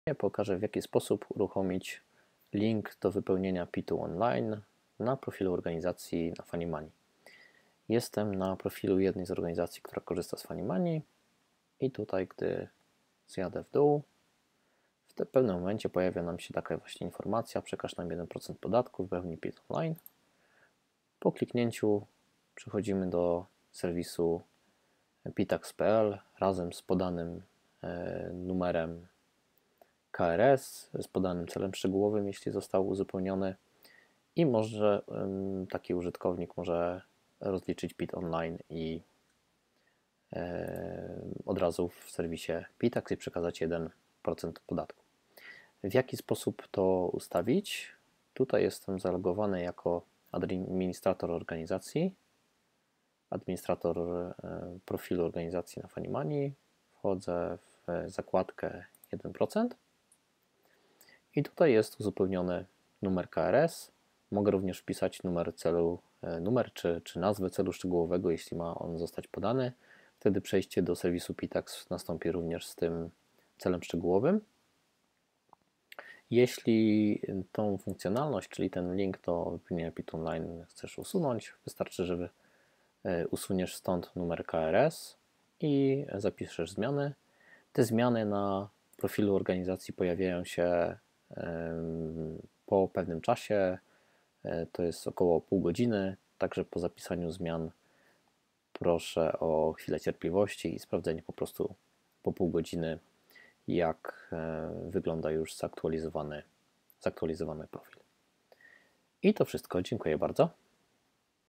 Narzędzia FaniMani - 1% podatku z serwisem Pitax.pl
Na profilu organizacji w serwisie FaniMani.pl można uruchomić link do serwisu Pitax.pl wraz z KRS organizacji i celem szczegółowym. Na tym filmie pokazuję jak to działa oraz jak można taki link w FaniMani skonfigurować.